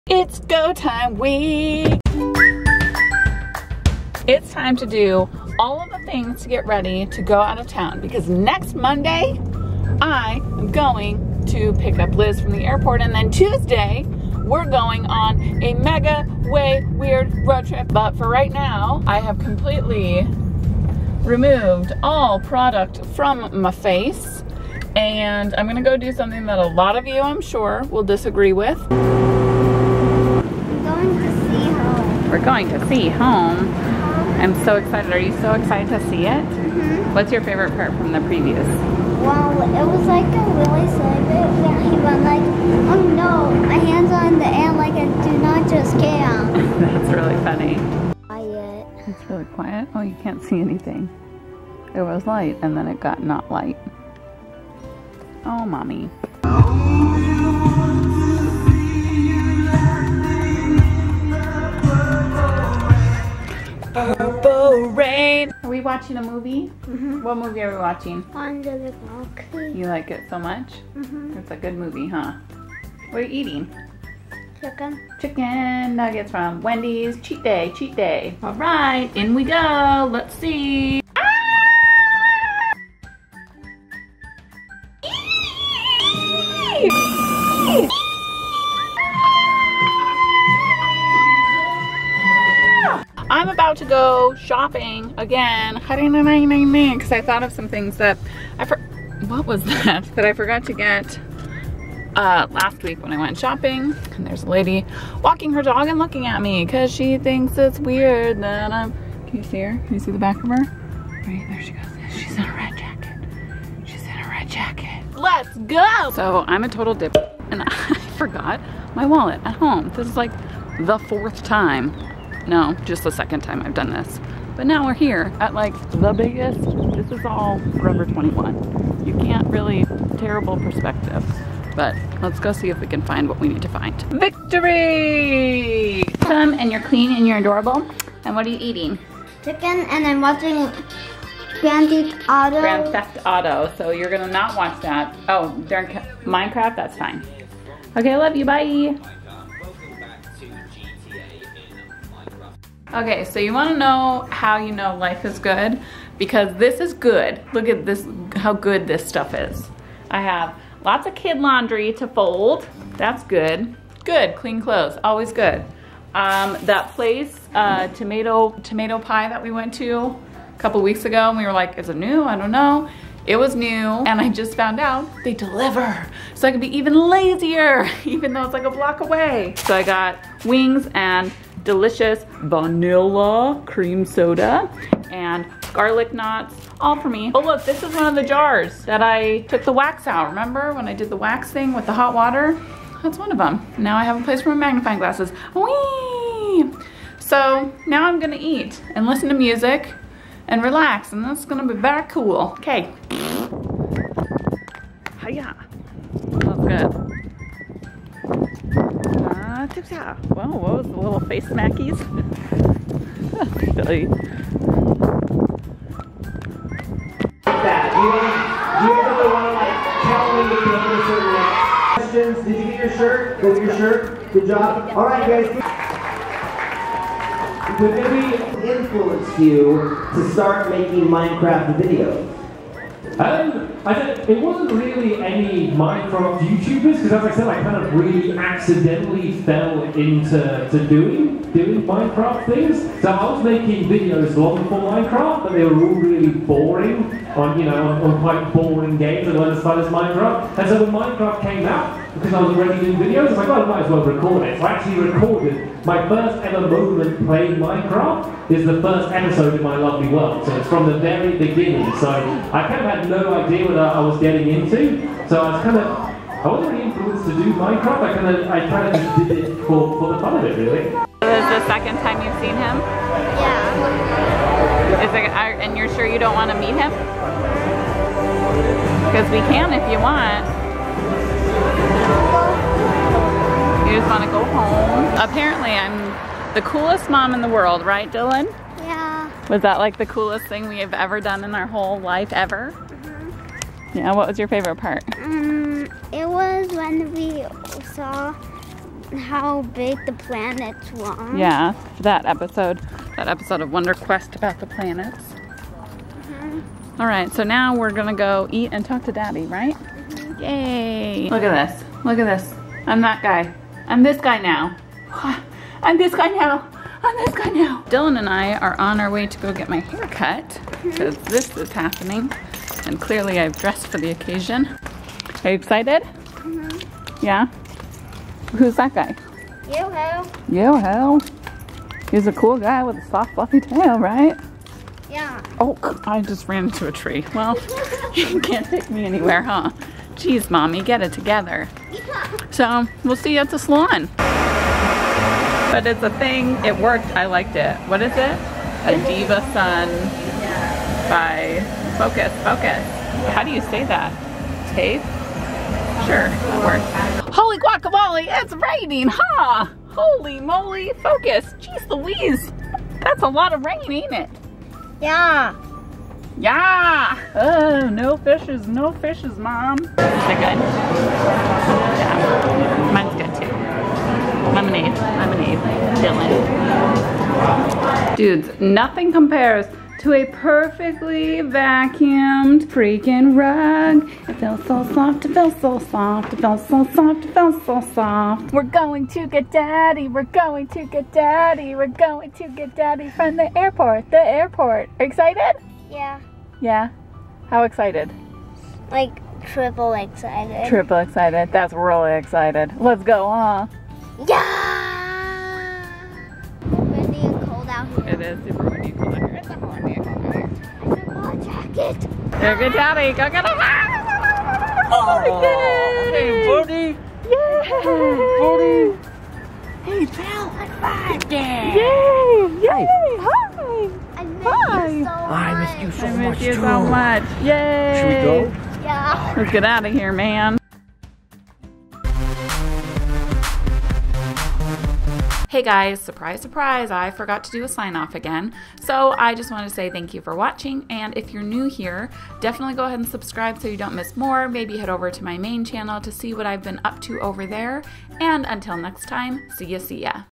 MEETING STAMPY // Jessi Sanfilippo
There was a boat load of stuff to get done before my trip to Arizona all while Steve being out of town for work the whole time and fun stuff like going to YouTube Space LA to meet Stampy and TheCommonPeople as they launched their new show, WonderQuest. Dylan was adorably so excited and nervous. I'm also realizing as I powered through this edit that HOLY MOLY, JESS! I need to take care of my skin and my body. No more late night chicken wings and garlic knots for this girl. YEESH! P.S. I missed you nerds. I'm never letting footage pile up like this ever again.